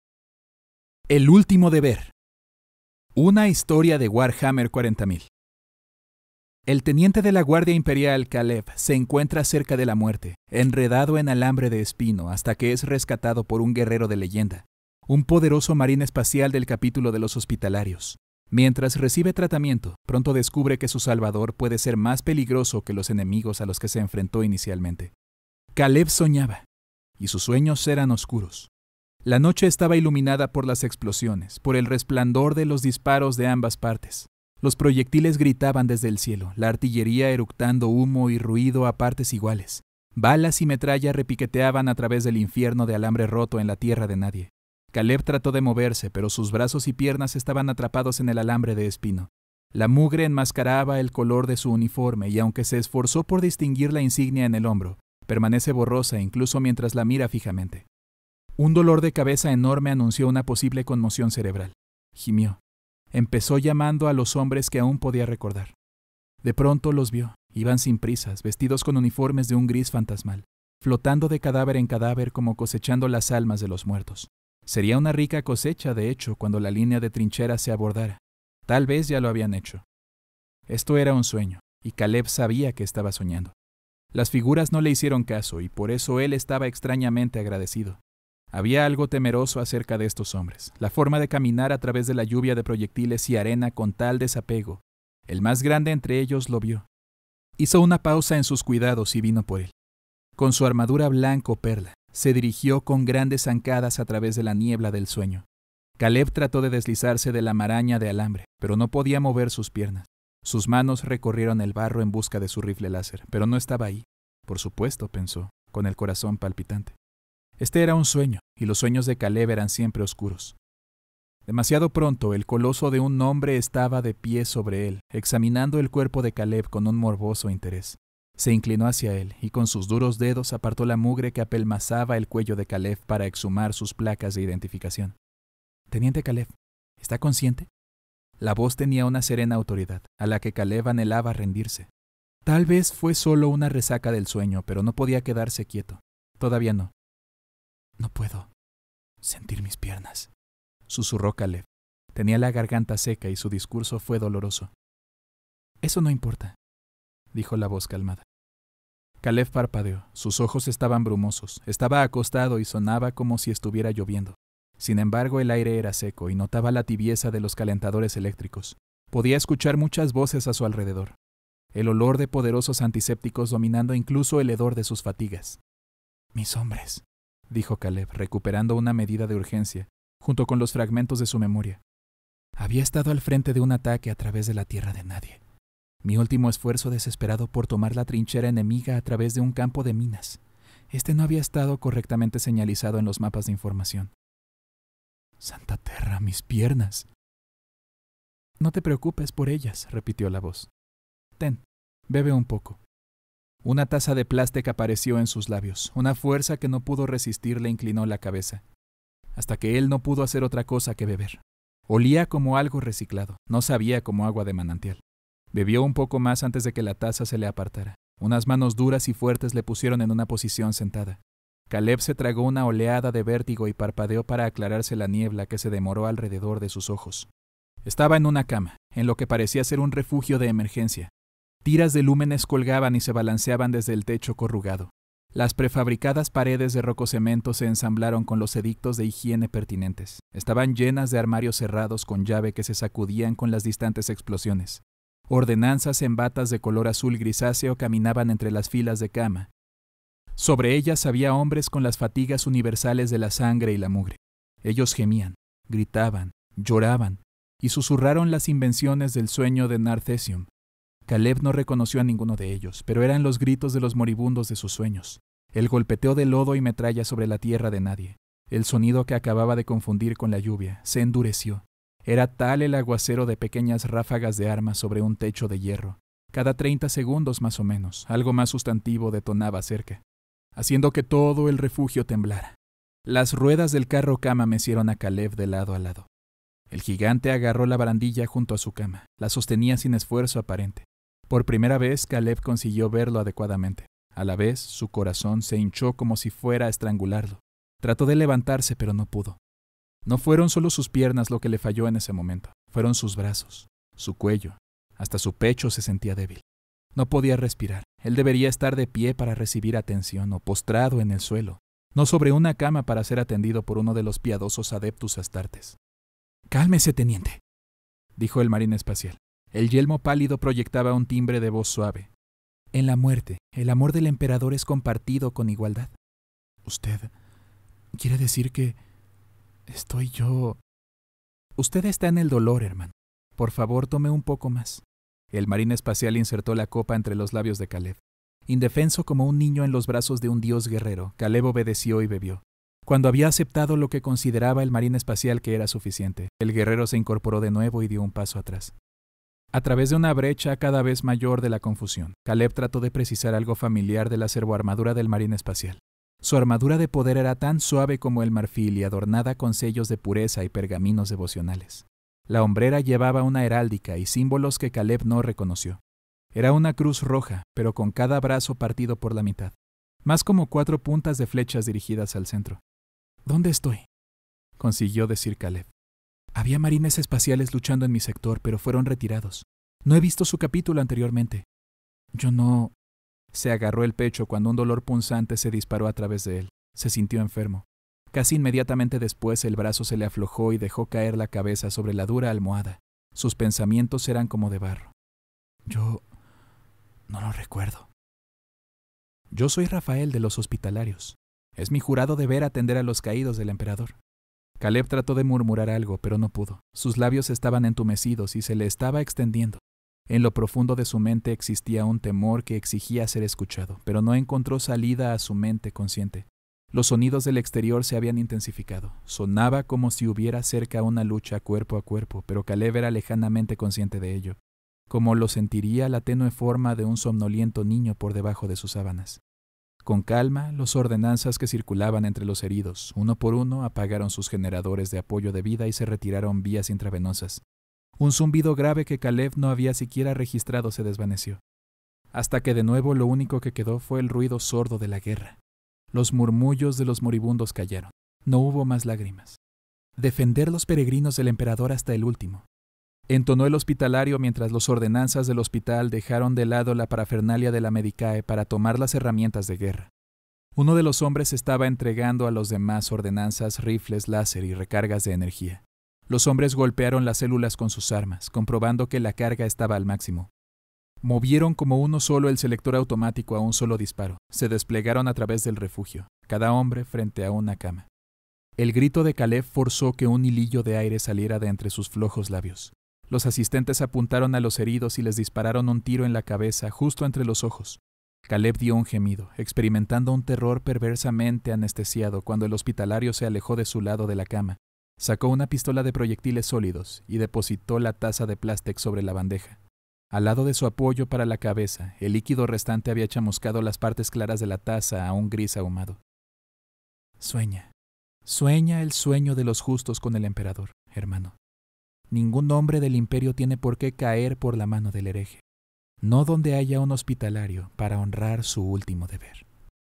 El Último Deber [Una Historia de Warhammer 40000]
El teniente de la Guardia Imperial Caleb yace al borde de la muerte, enredado en un alambre de púas, hasta que es rescatado por un guerrero de la leyenda, un poderoso Marine Espacial del Capítulo de los Hospitalarios. Tomado para recibir tratamiento, pronto descubre que su salvador puede ser más peligroso que los enemigos a los que se enfrentó originalmente.